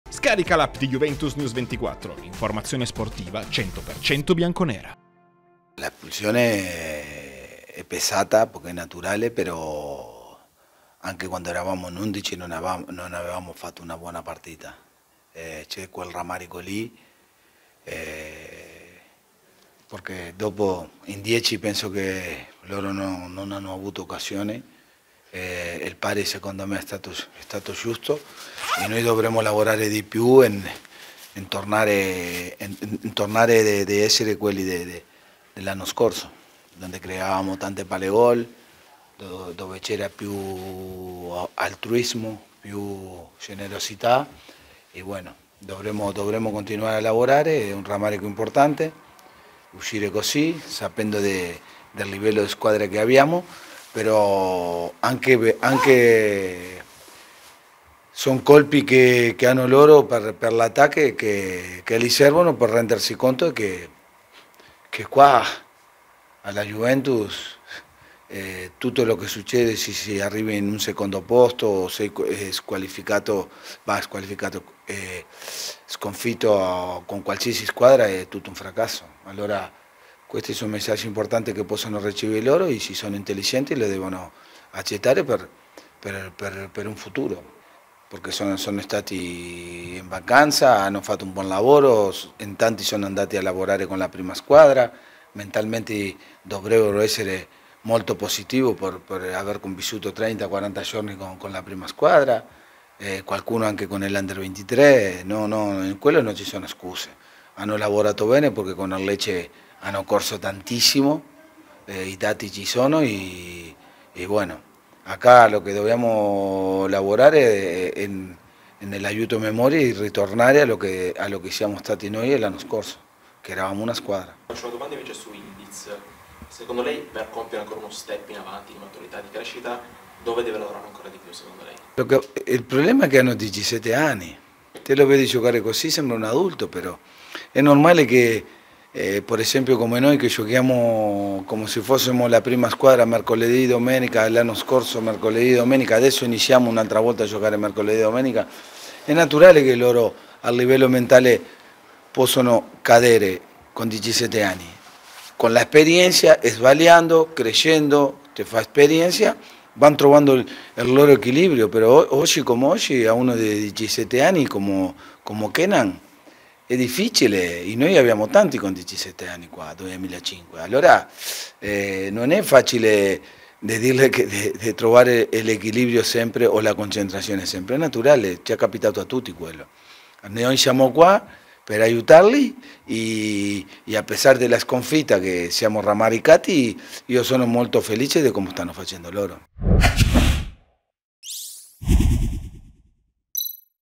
Montero post Juve Primavera-Napoli 3-5 d.c.r (Coppa Italia): "Torniamo quelli del 2022"
L' allenatore della Juventus Primavera  al termine di Juventus Primavera-Napoli di Coppa Italia.